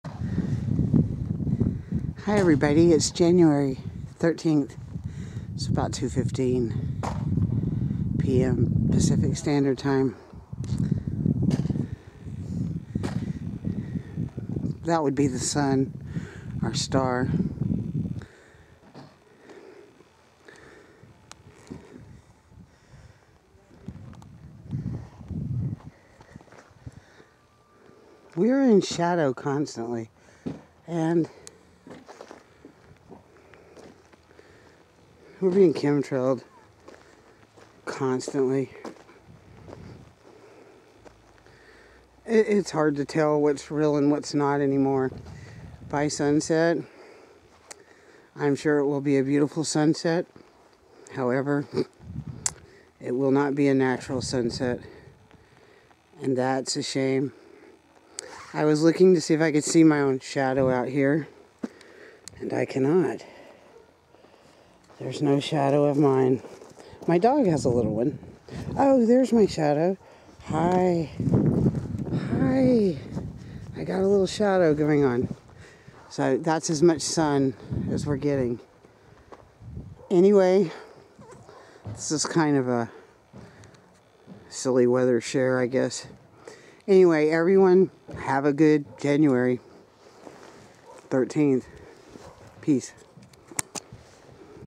01-13-2013_ Washington State Chemtrail report
Its cold and we are being sprayed like roaches I would love to see a blue sky and sunshine